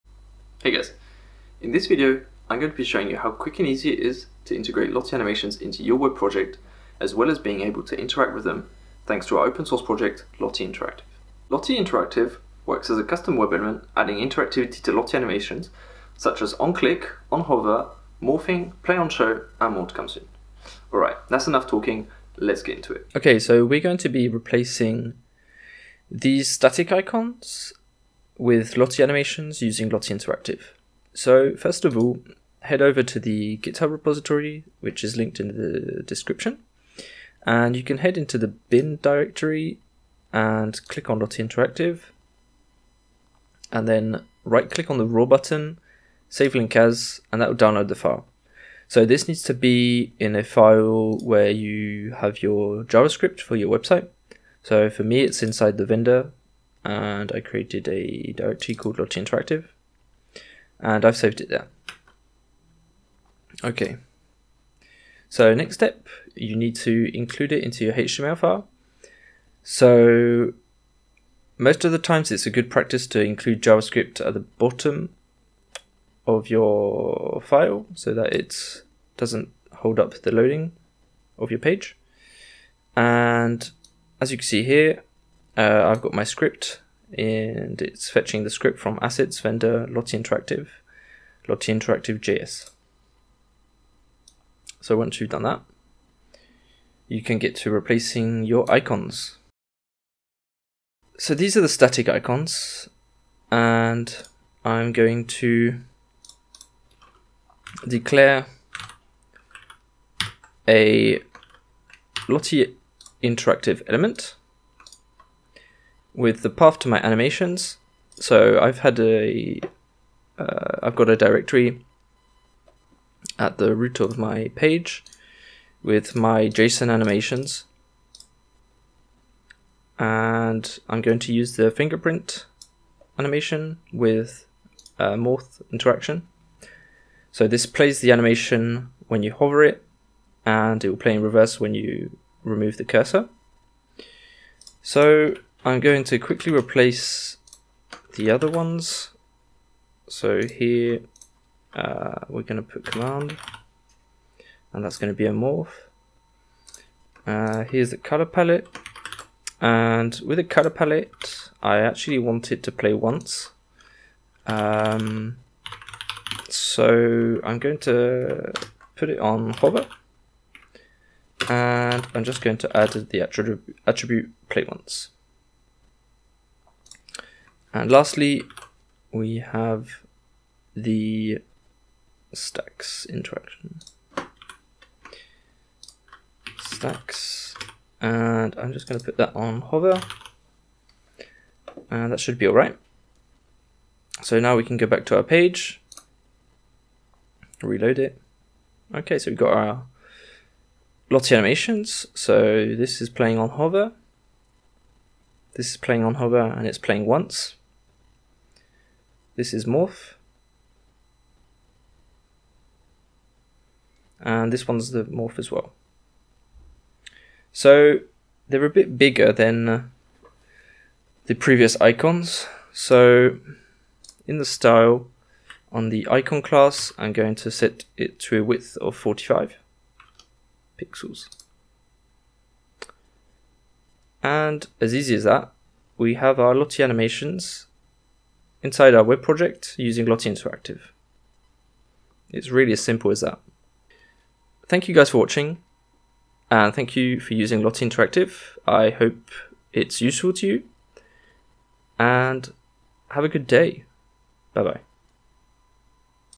Interactivity for Lottie with Lottie-Interactive
Lottie-interactive is a custom web element extending the original Lottie web player by adding interactivity to Lottie animations. Lottie-interactive
is the easiest way to integrate Lottie animations and quickly get up and running with interactions that will surprise and wow your users.